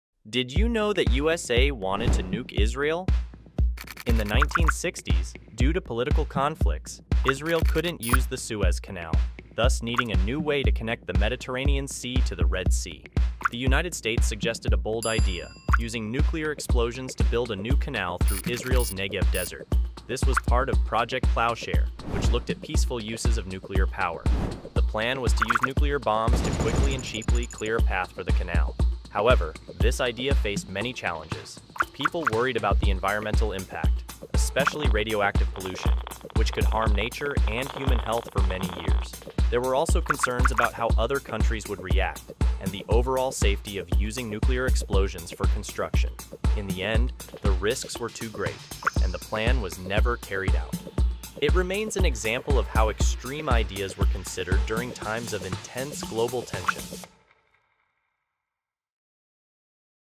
How the USA Wanted to Nuke Israel
In the 1960s, Israel needed a new way to connect the Mediterranean Sea to the Red Sea because they couldn't use the Suez Canal due to political conflicts. The United States suggested a bold idea: using nuclear explosions to build a new canal through Israel’s Negev Desert. This was part of "Project Plowshare," which looked at peaceful uses of nuclear power.